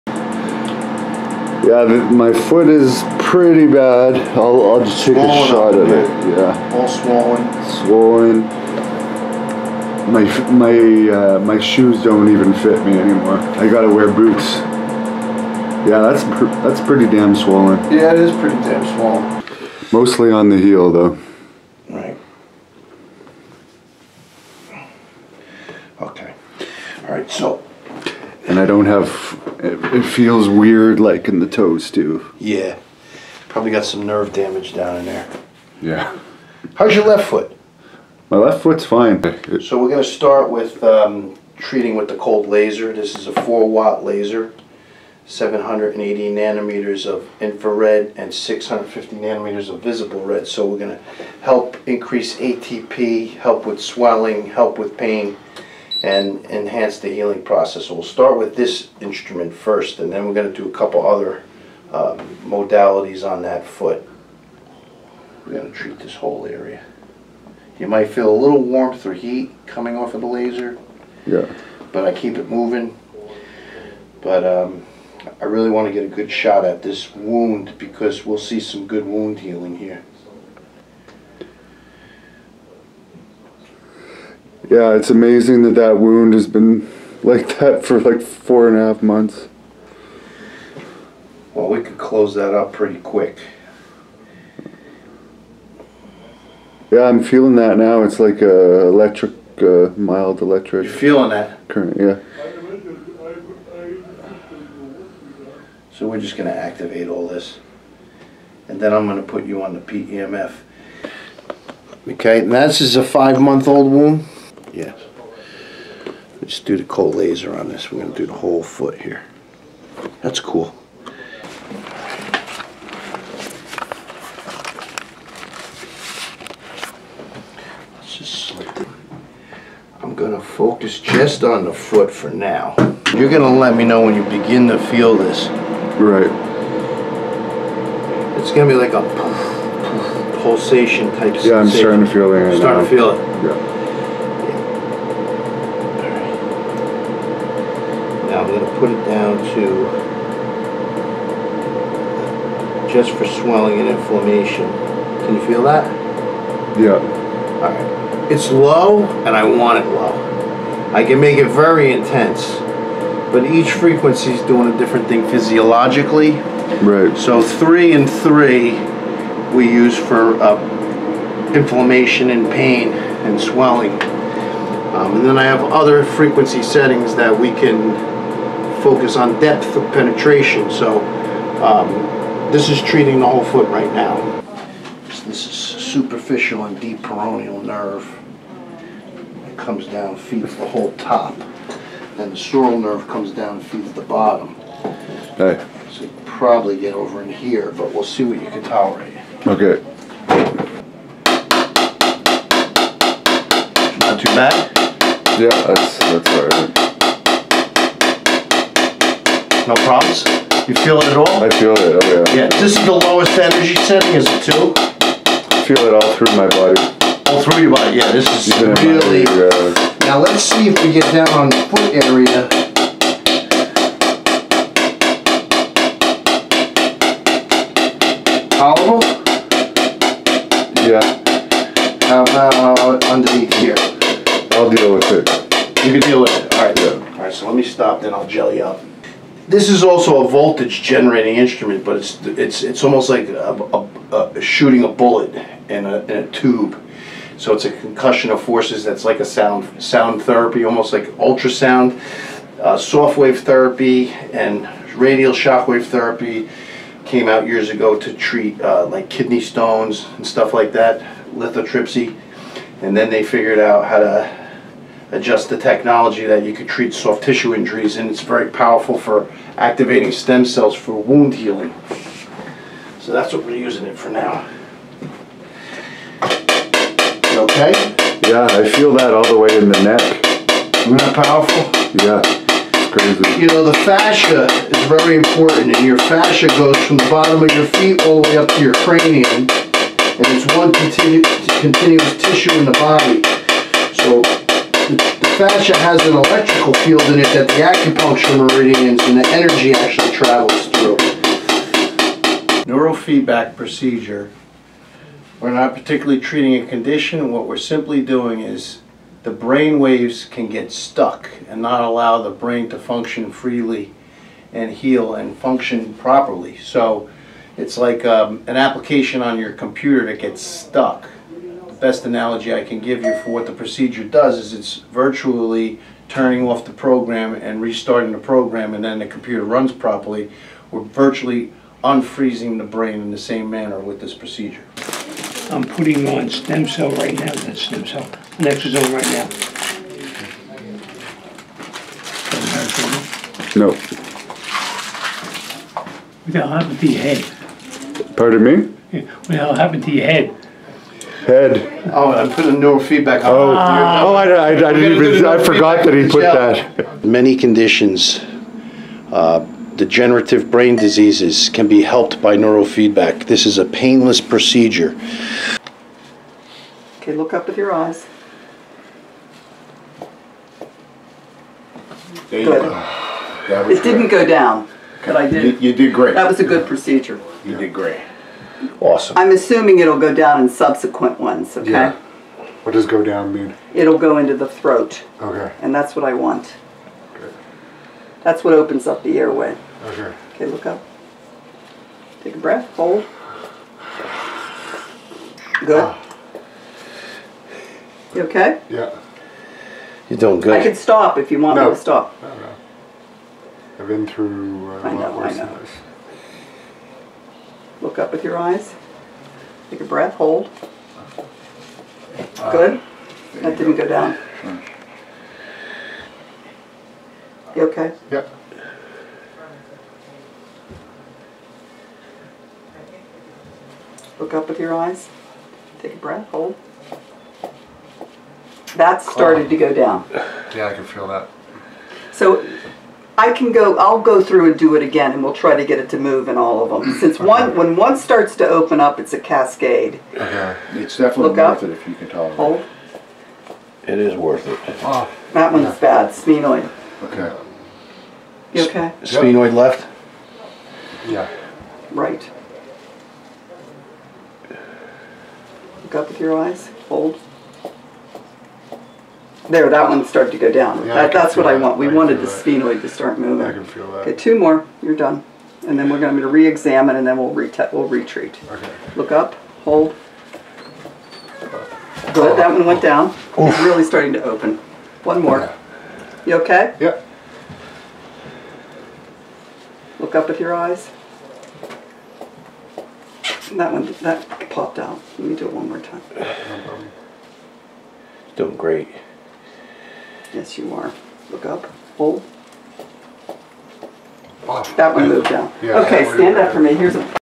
Hannibal's Transformation Through Foot & Brain Treatments
Hannibal Gets Foot & Brain Function Treatments at infinate Wellness

Thanks to Integrated pain management in New Jersey.

Follow The Hannibal TV on Instagram, Facebook, YouTube & Twitter  @TheHannibalTV